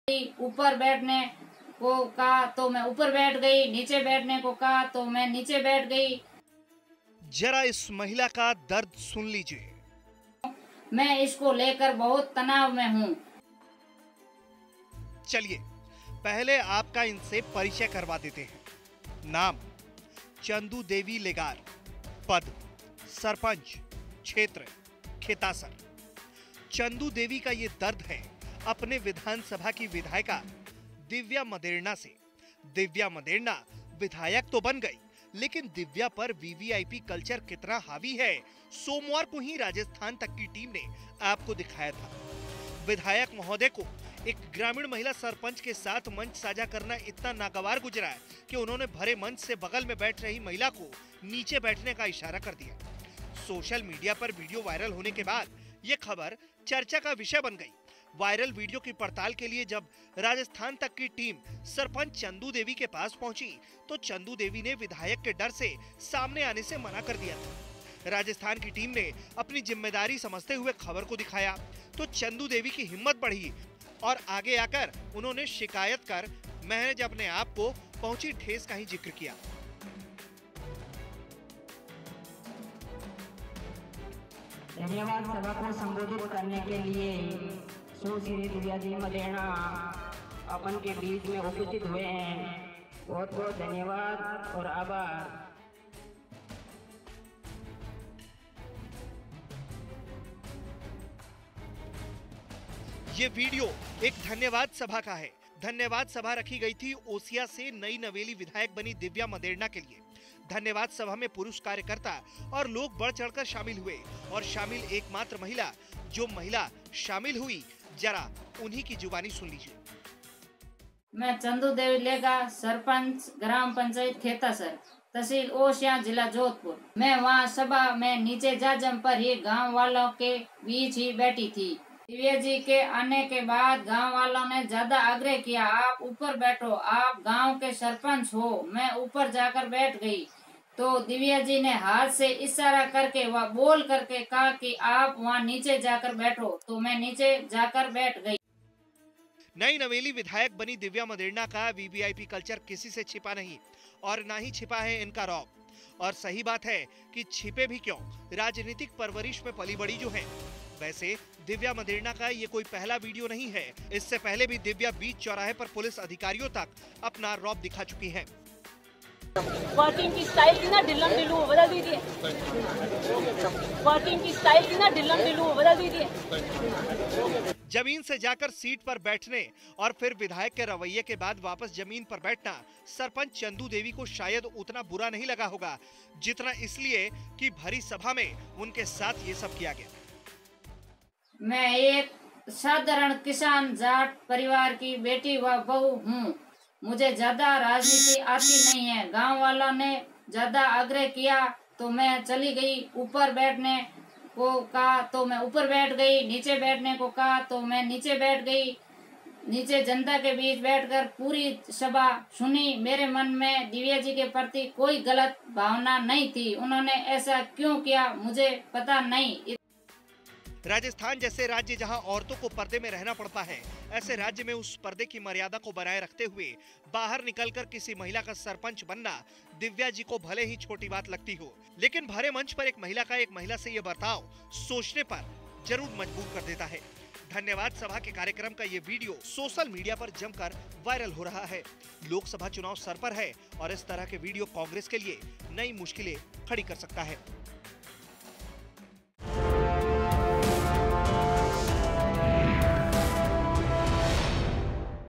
'DIVYA MADERNA की दुत्कार वाली सरपंच' !| Rajasthan Tak
JODHPUR की ओसियां सीट से विधायक दिव्या का वीवीआईपी रूप सरपंच महिला को उतारा मंच से